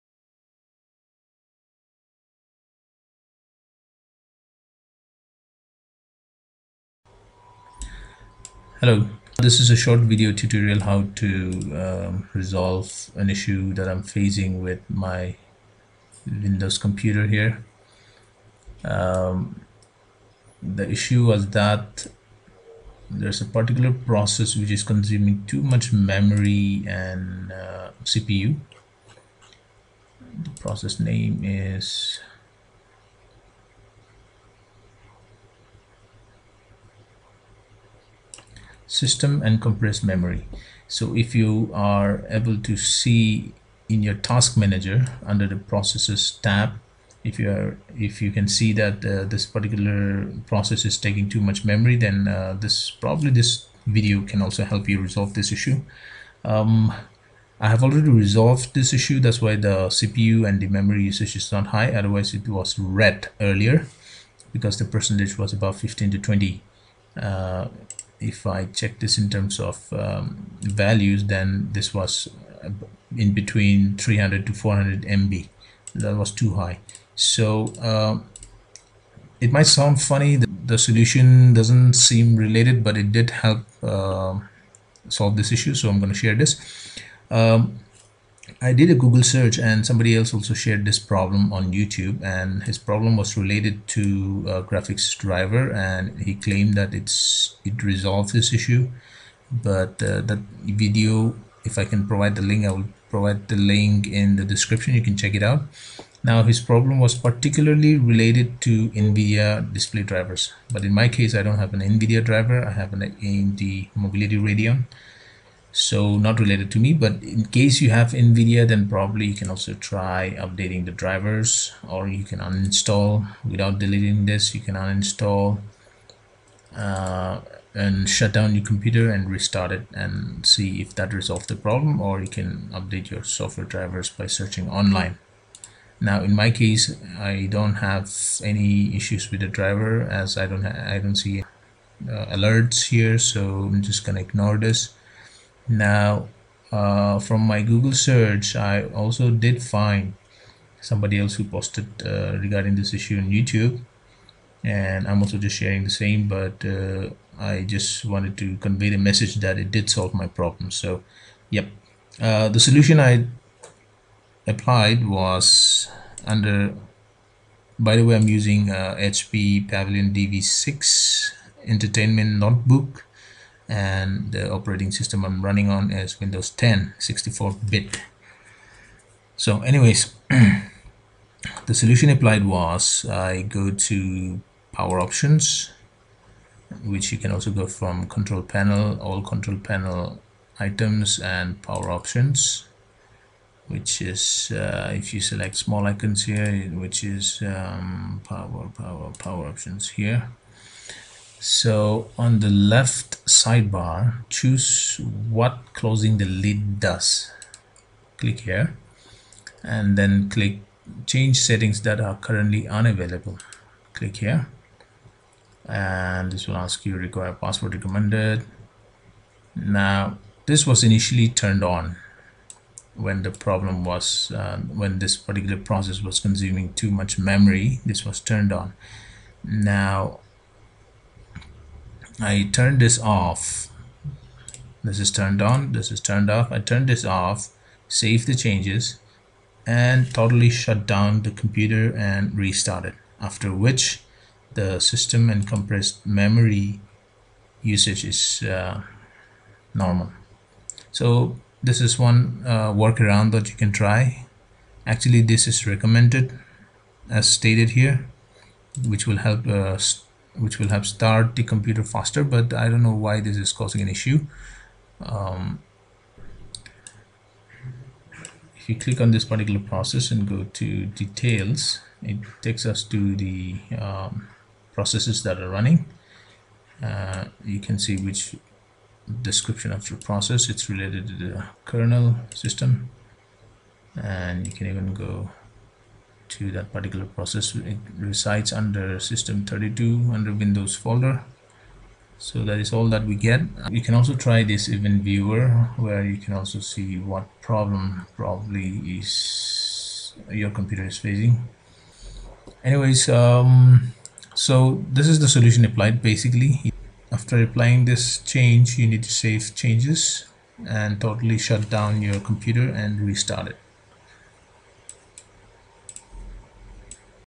Resolved: System & Compressed Memory process consuming high cpu and memory Windows 10
System & Compressed Memory process consuming high cpu and memory Windows 10 64 bit. Please note that this solution may not apply in your case.